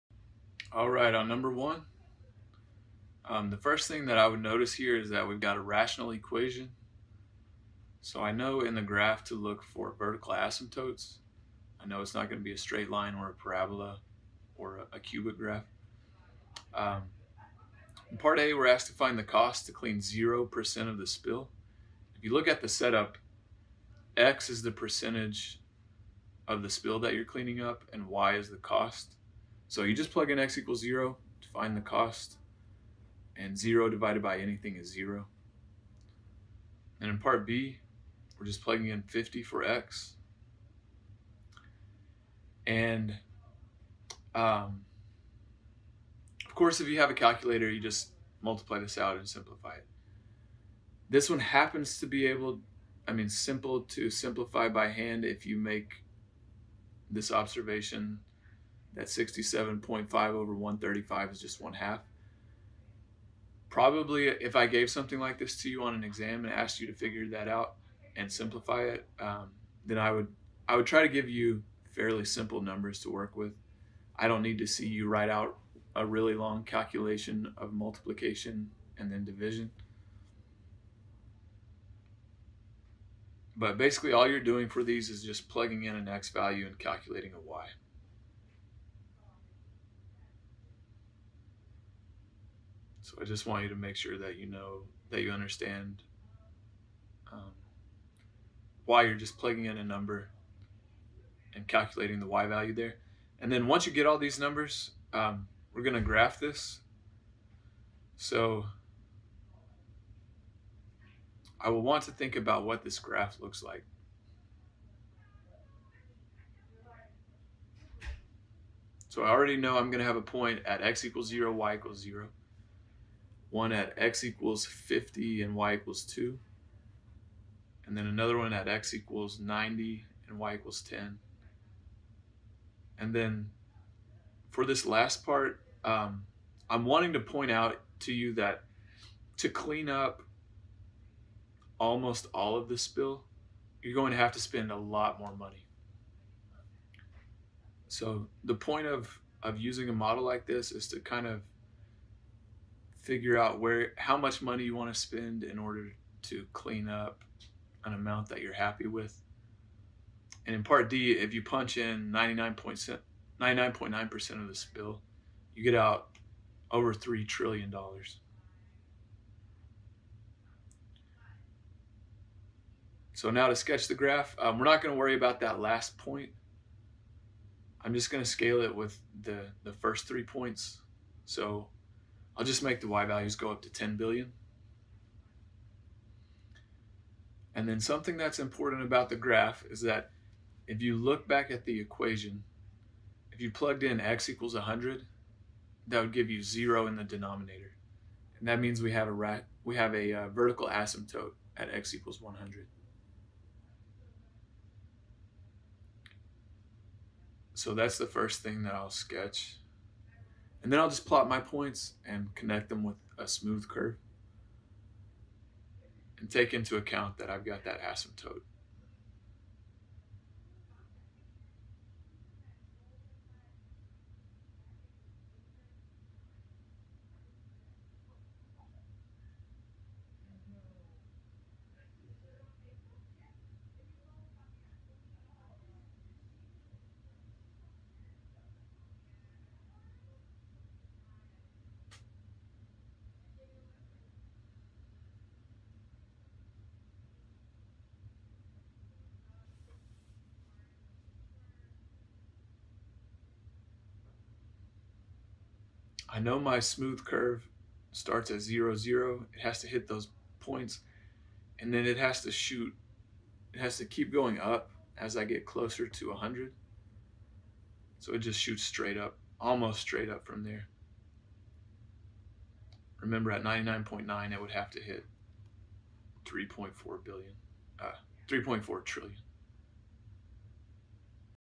College Algebra HW4 #1
Analyzing a rational model for an oil spill